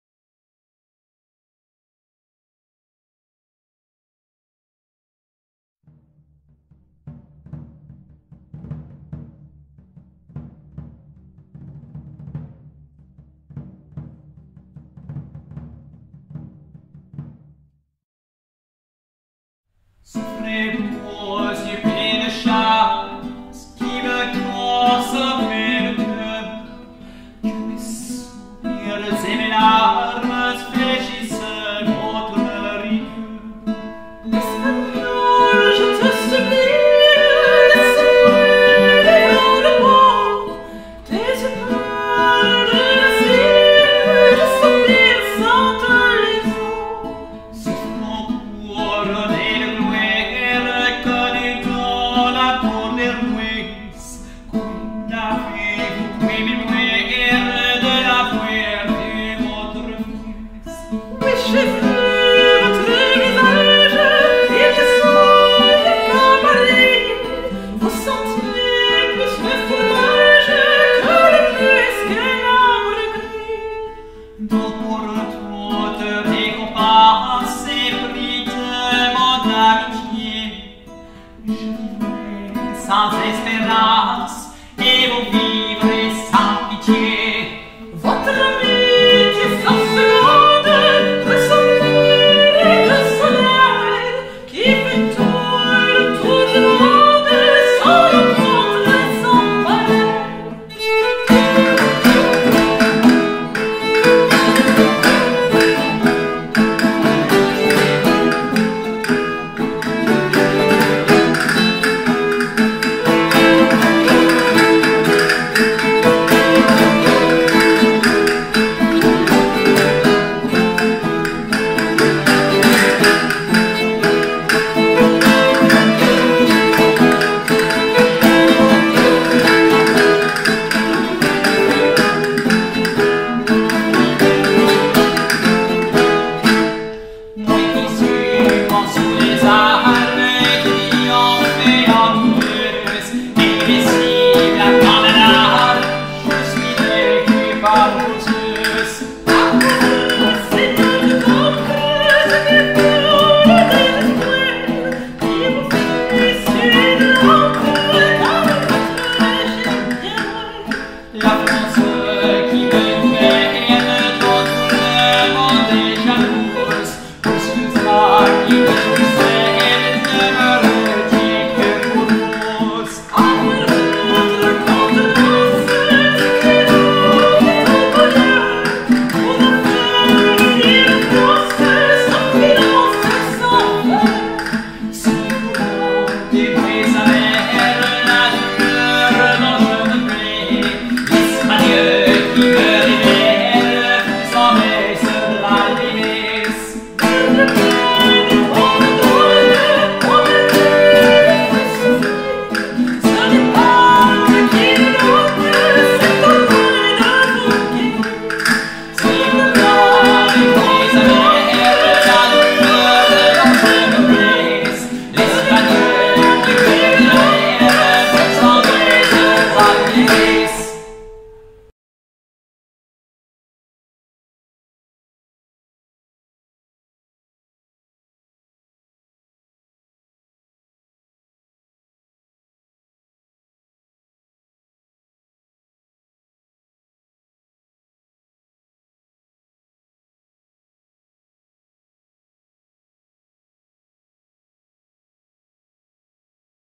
Renaissance ?! Baroque Music and Song in A royal Castle Chateau in France (2017)
Renaissance ? Baroque Music and Song in A royal Castle Chateau in France (2017) .Enjoy Viola guiatr castanets and two singers play this ancient song in a Royal court in a king castle.17th century traditional music with old instruments.

More Youtube videos for you